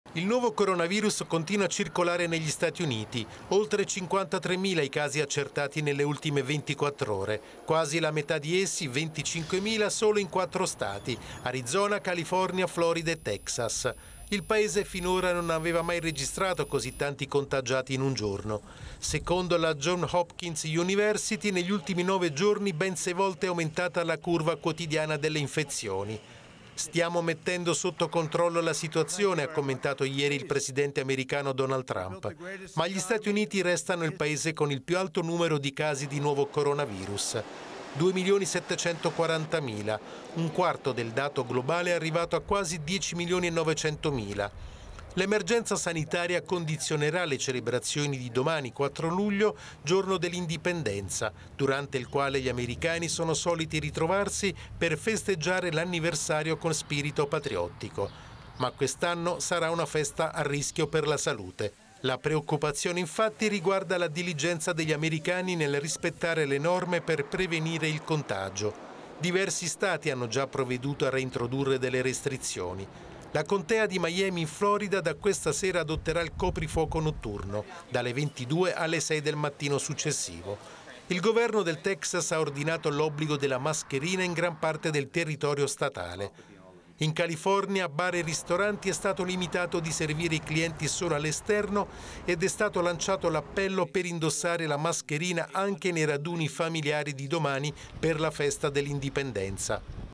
Coronavirus, Usa verso chiusure per virus
Negli Stati uniti il giorno più buio: record di contagi. Gli Stati si preparano a chiusure. Una mano a Trump arriva dal calo della disoccupazione. Il servizio di Maurizio Di Schino